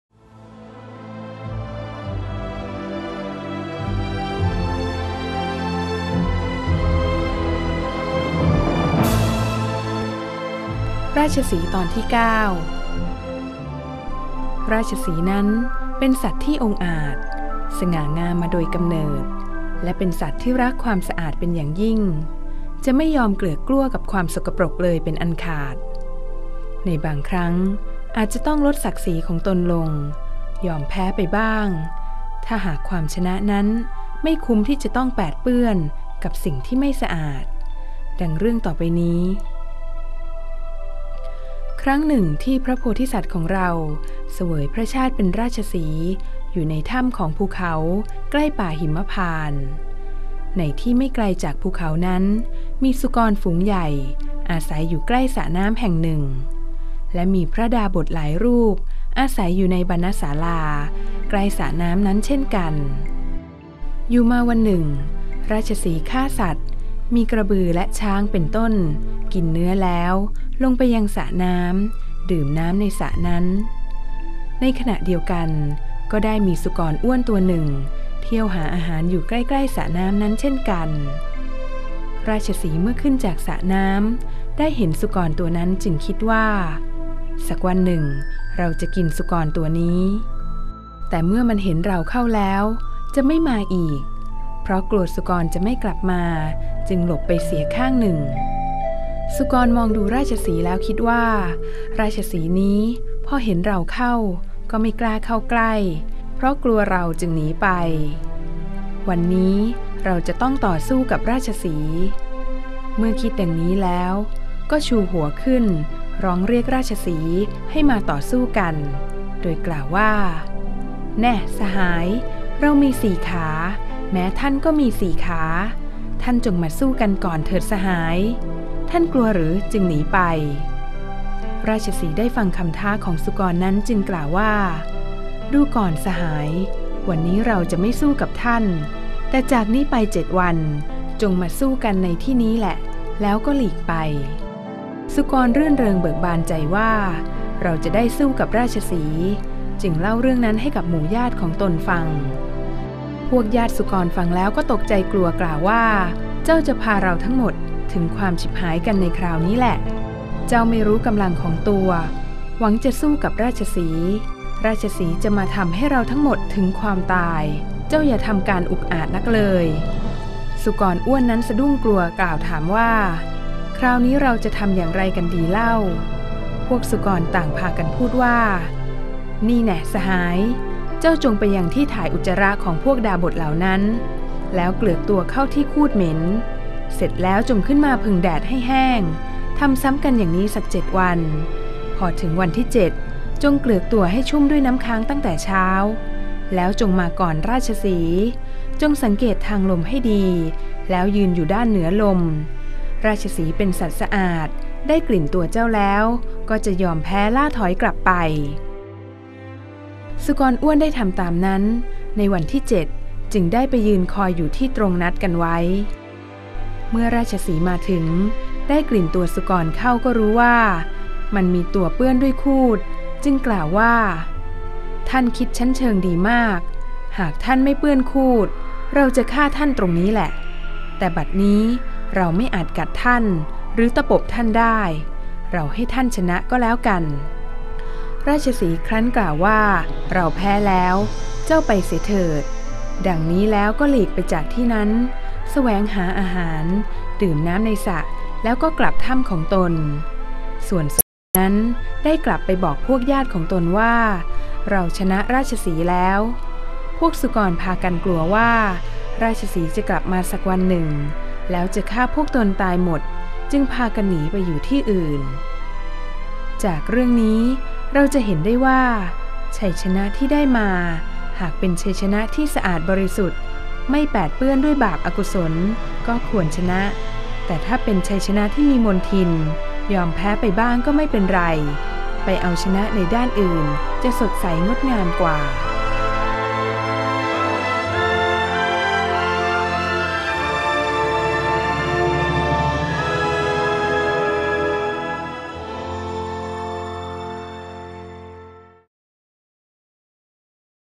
ตอนที่ 9 ราชสีห์รักความสะอาดเป็นอย่างยิ่ง
ราชสีห์ อาศัยอยู่ในป่าหิมพานต์มี 4 ตระกูล
ได้แก่ ติณราชสีห์ กาฬราชสีห์ ปัณฑุราชสีห์ และไกรสรราชสีห์